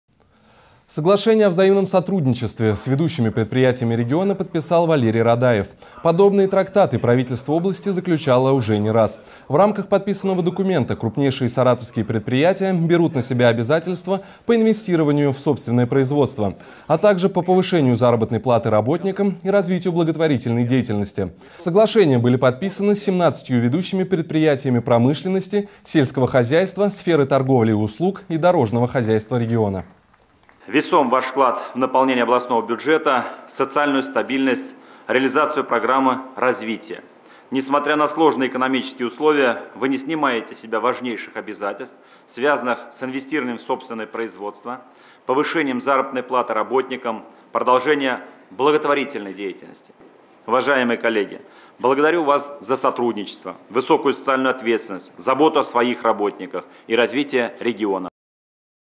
Валерий Радаев подписали соглашение с ведущими предприятиями
Подобные трактаты Правительство области заключало уже не раз. В рамках подписанного документа крупнейшие саратовские предприятия берут на себя обязательства по инвестированию в собственное производство. Материал вышел в эфир 1 апреля 2015 года.